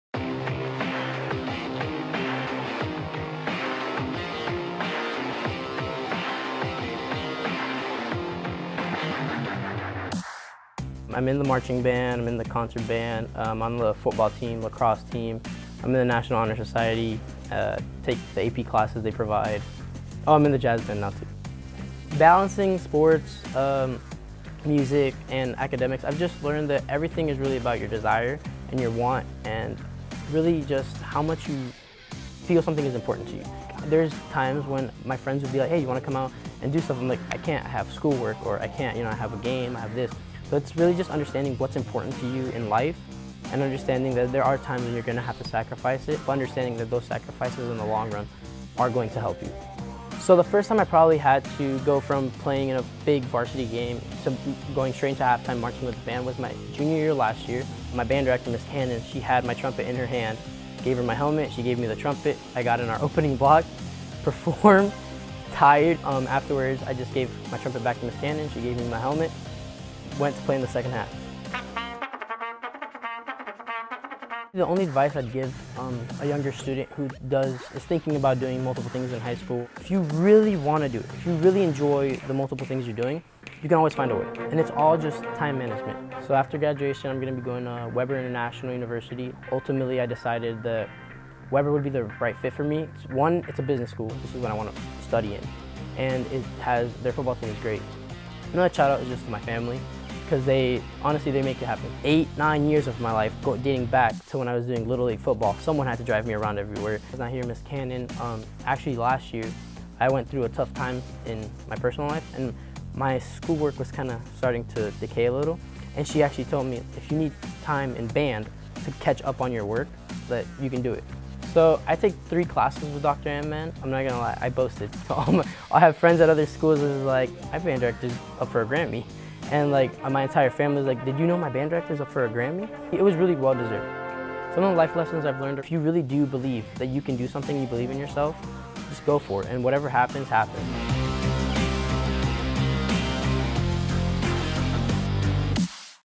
OCPSGrads Freedom HS - Reynold Rey
His passion for music, football and academics forced Reynold Rey to learn time management. With a schedule full of advanced placement courses, Reynold had to balance the coursework in those classes with varsity football practice and marching band rehearsals, and learned how to quickly exchange his helmet for his trumpet for the half-time shows!